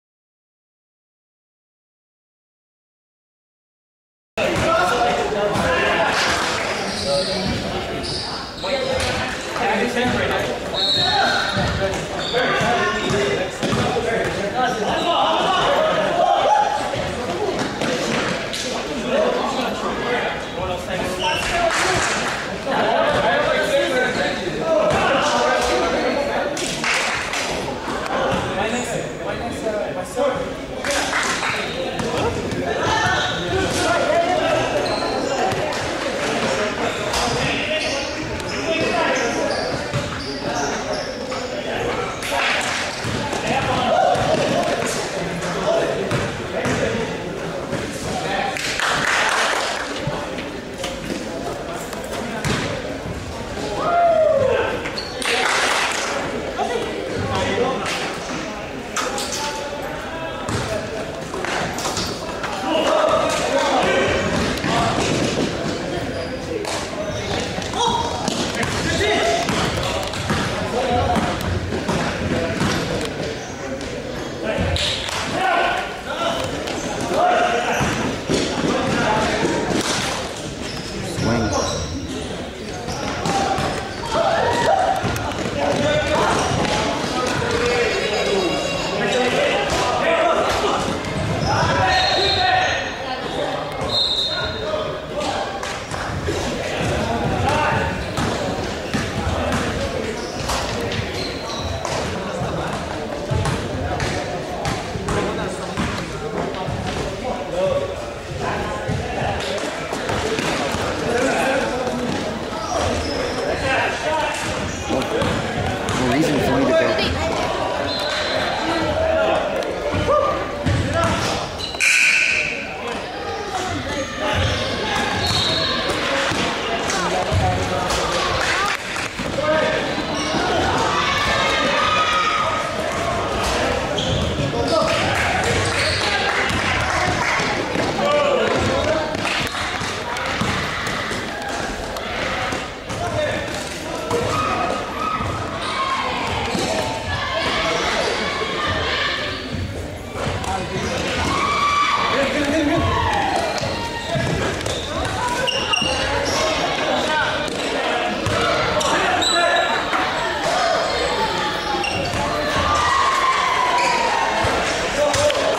LVABL - Team-X 11/12 Highlights
Team-X vs. Krazy Island Boys.
November 12, 2008
YMCA Meadows. Rubber Court.

for more coverage.

Las Vegas Asian Basketball League